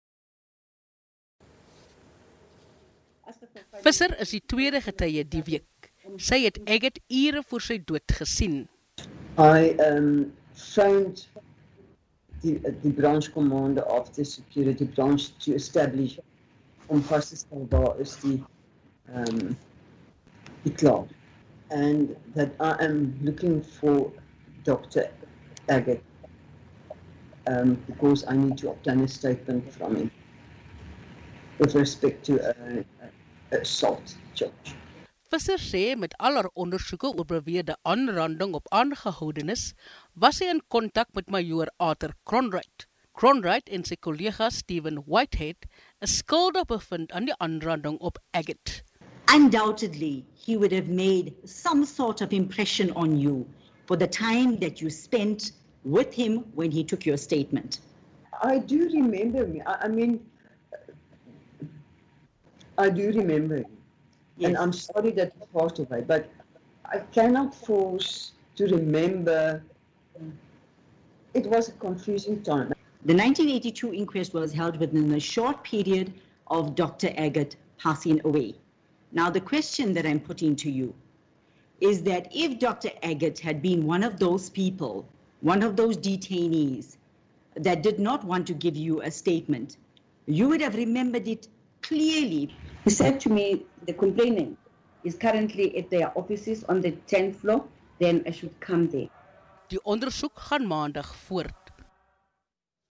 Getuienis oor aanranding | Neil Aggett
'n Voormalige sersant, het getuig, in die ondersoek, na die dood van die anti- apartheid aktivis, Neil Aggett, wat sy, ure voor sy dood, gesien het. Dit was die taak, van Aletta Visser, om die aanklagte, teen haar kollegas, by die destydse John Vosterplein polisie stasie, in Johannesburg, te ondersoek. Visser sê in die verklaring, wat sy, van dr. Aggett gekry het, is hy die oggend van 4 Febuarie 1982, aangerand.

Vir meer nuus, besoek sabcnews.com en ook SABCNews SABCNuus COVID19News op sosiale media.